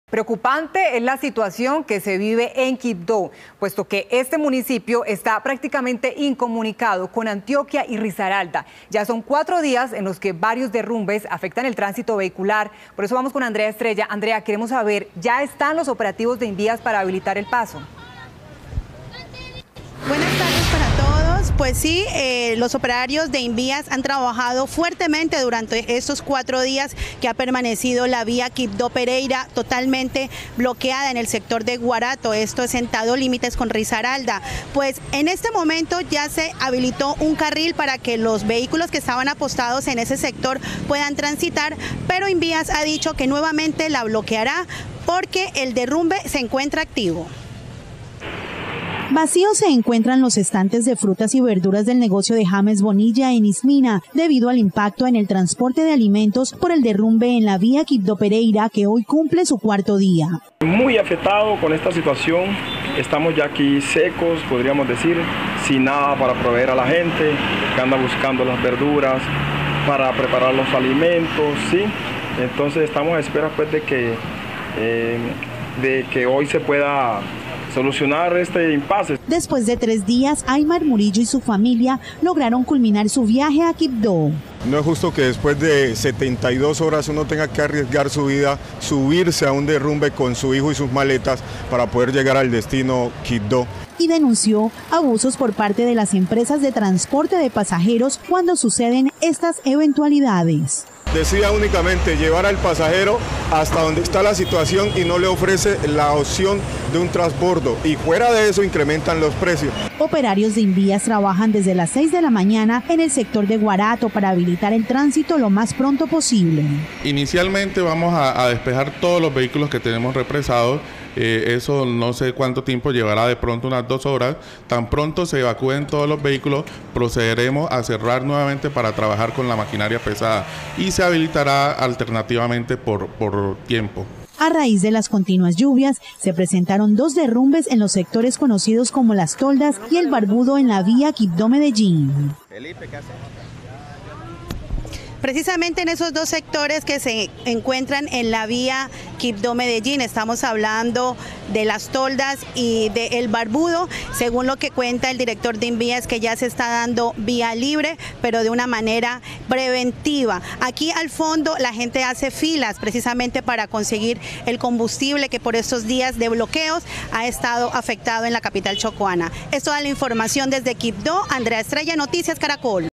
Quibdó está incomunicado con Antioquia y Risaralda debido a derrumbes en las carreteras
Operarios de Invás están trabajando fuertemente para restablecer la circulación vehicular.